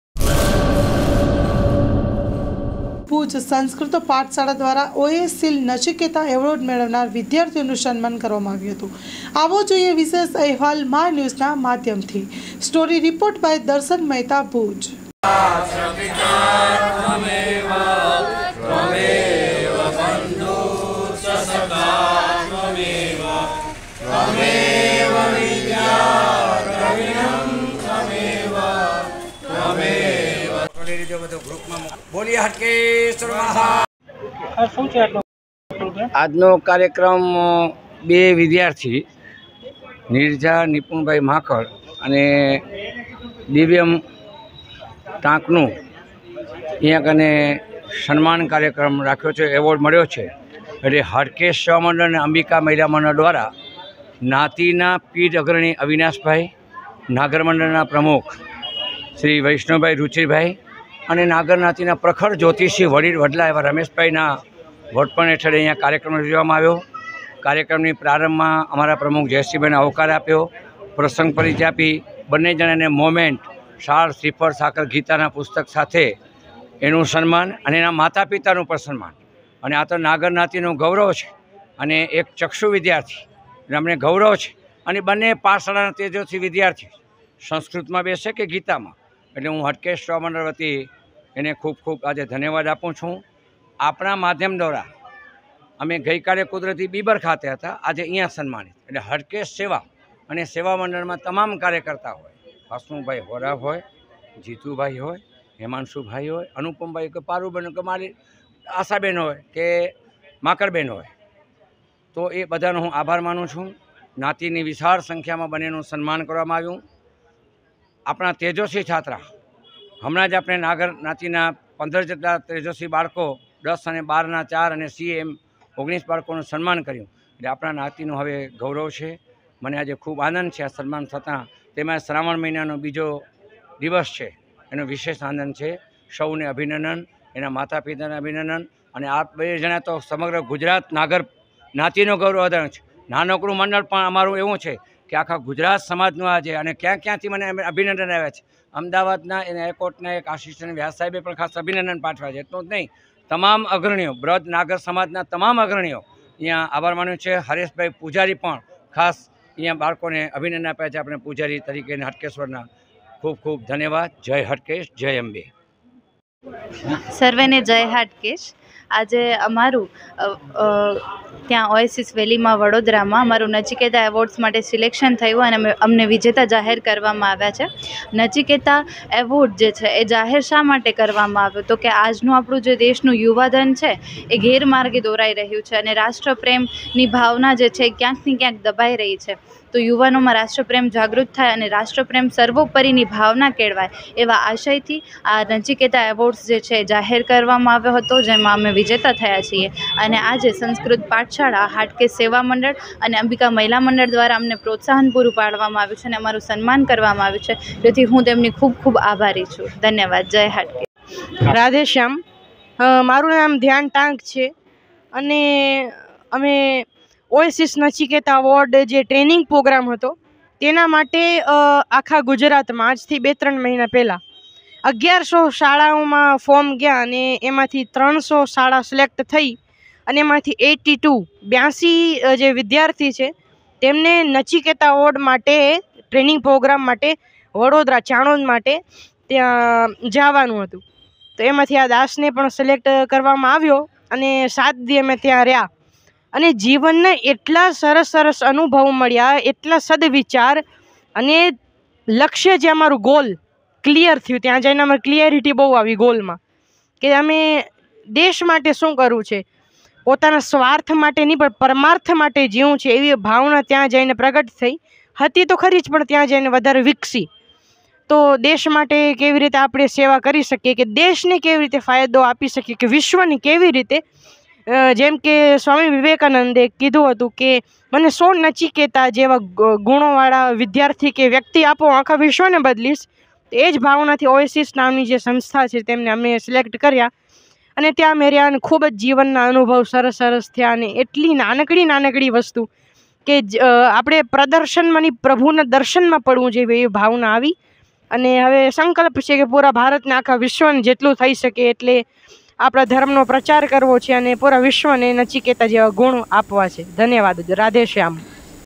ભુજ સંસ્કૃત પાઠશાળા દ્વારા ઓએશીલ નચિકેતા એવોર્ડ મેળવનાર વિદ્યાર્થીઓનું સન્માન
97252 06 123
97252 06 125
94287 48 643